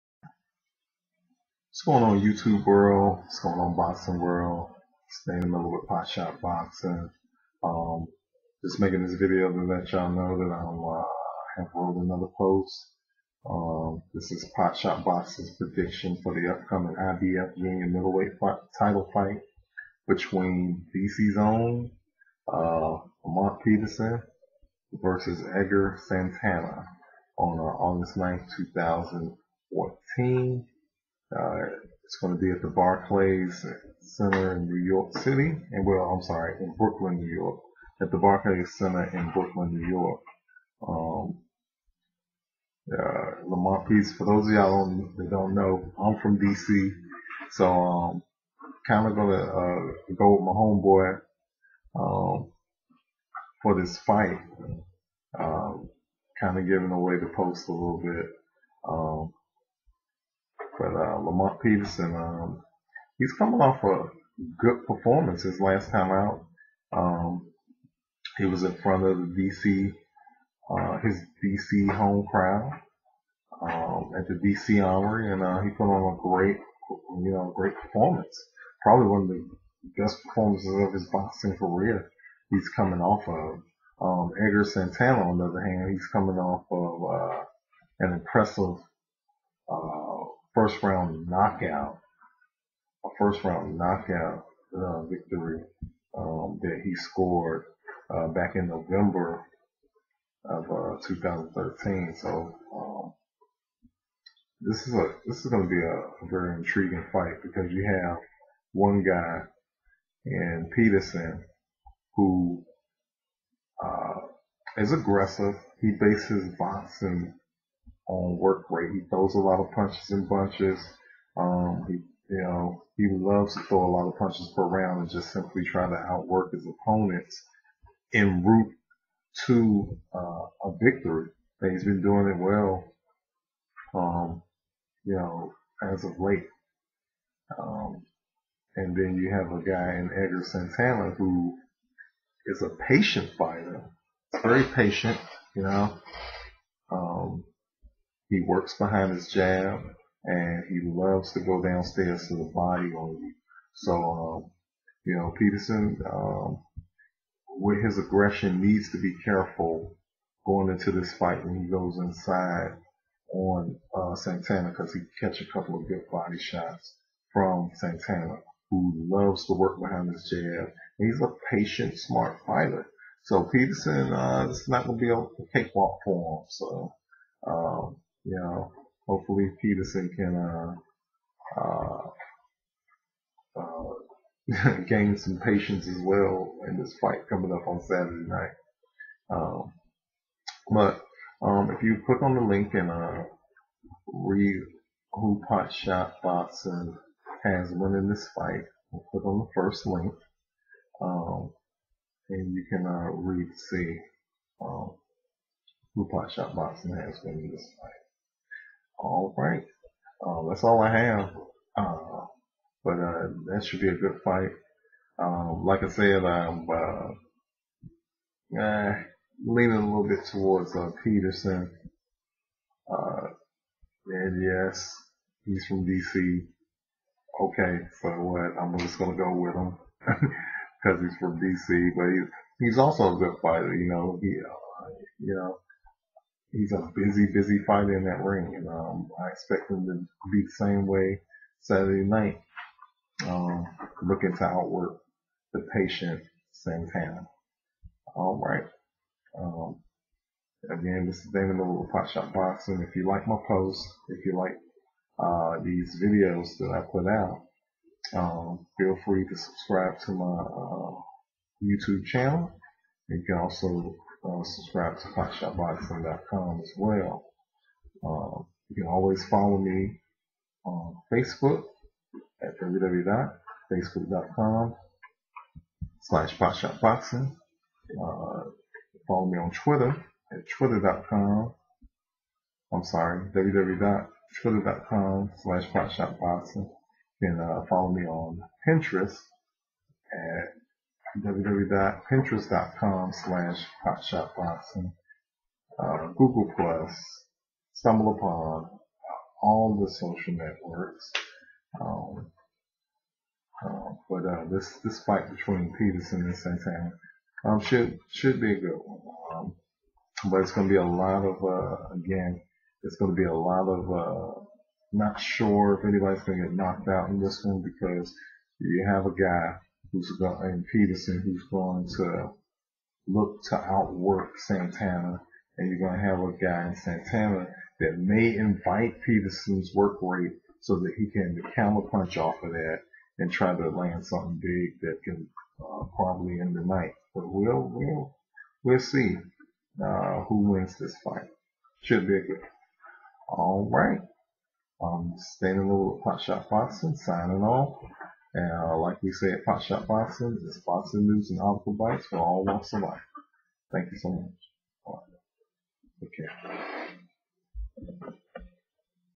Lamont Peterson vs. Edgar Santana Prediction
Potshot Boxing's (PSB) Prediction for the upcoming IBF junior welterweight title fight between Lamont Peterson and Edgar Santana!!

Need a CPA network? Try MaxBouty...